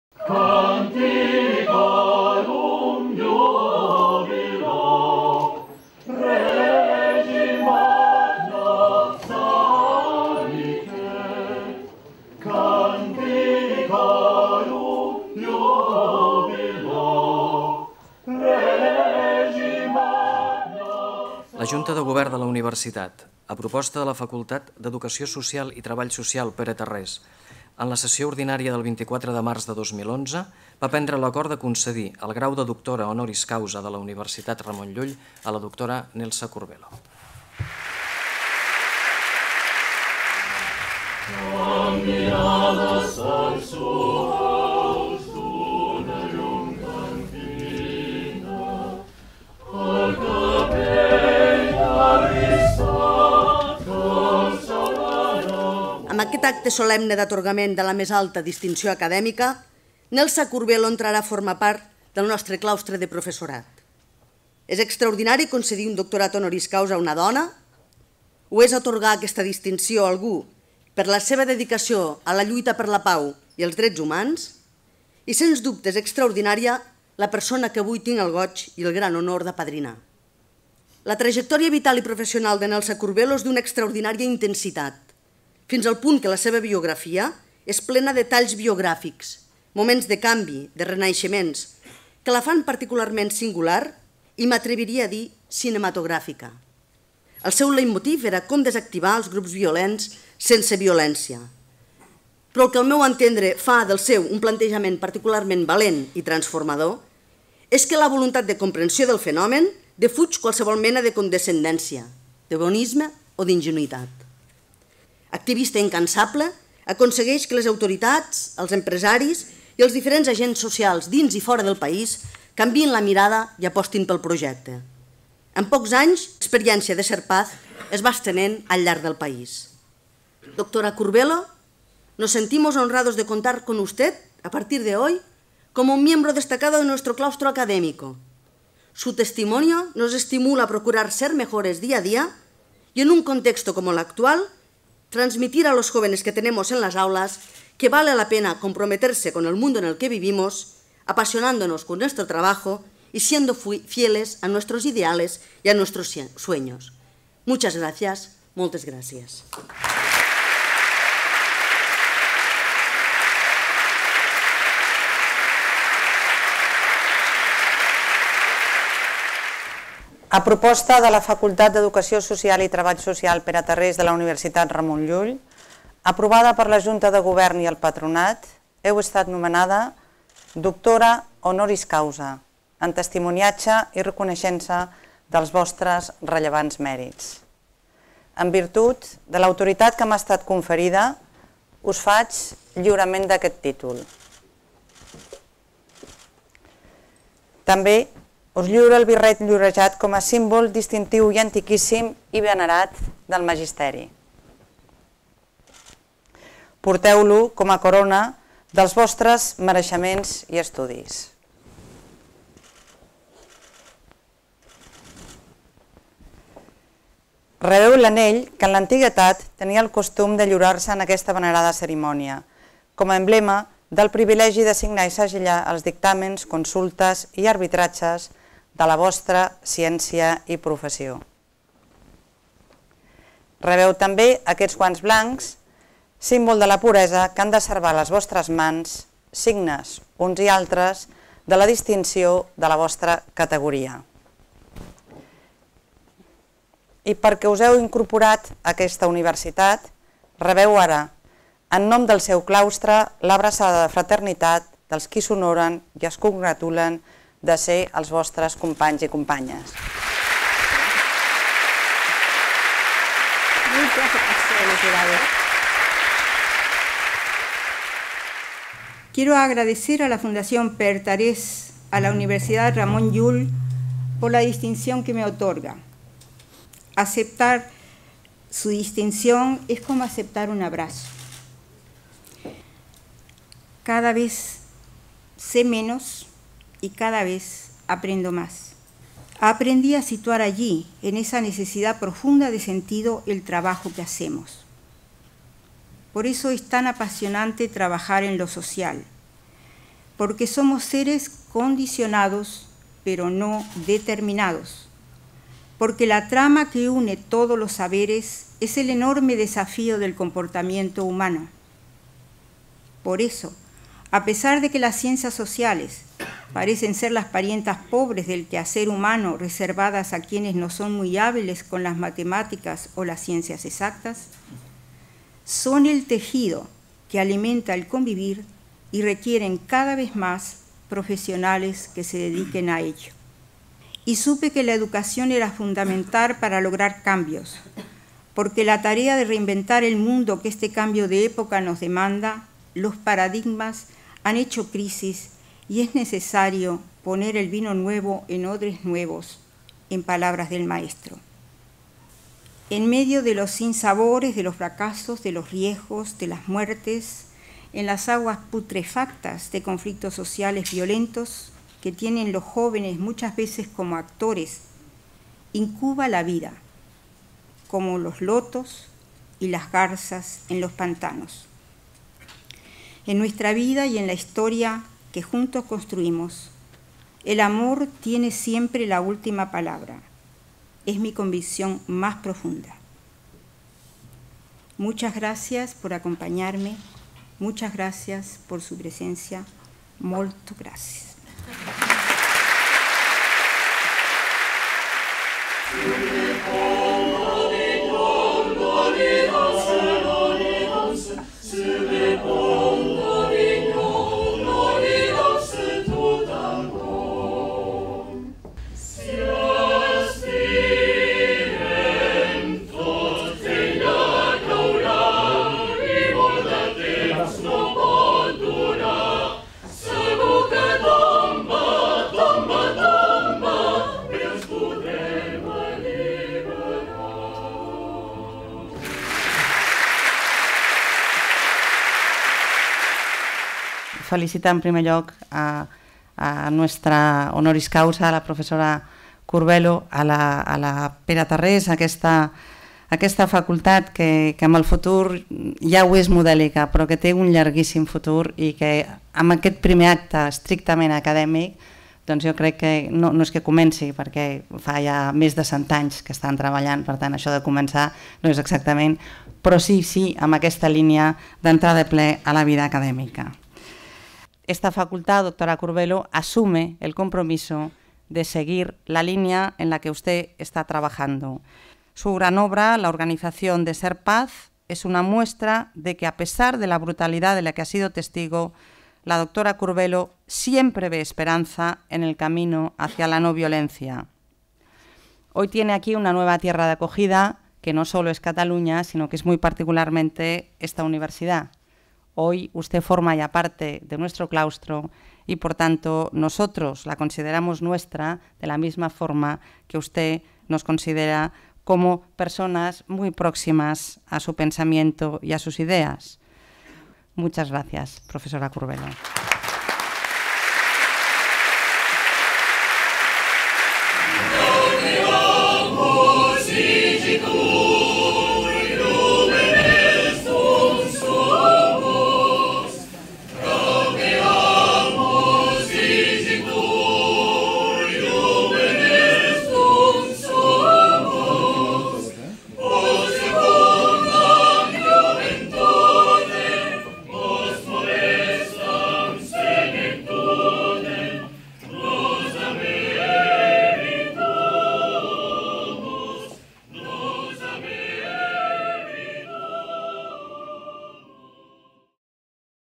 Fundació Pere Tarrés: Acte d'investidura de doctora honoris causa a la Dra. Nelsa Curbelo (resum)
Nelsa Curbelo, pedagoga experta en drets humans i noviolencia i directora de l'ONG Ser Paz, va ser investida com a doctora honoris causa per la Universitat Ramon Llull el 10 de febrer a l'Aula Magna de la Facultat d'Educació Social i Treball Social Pere Tarrés.
La Dra. Curbelo és la primera doctora honoris causa nombrada per la Universitat Ramon Llull a proposta de la Facultat d'Educació Social i Treball Social Pere Tarrés.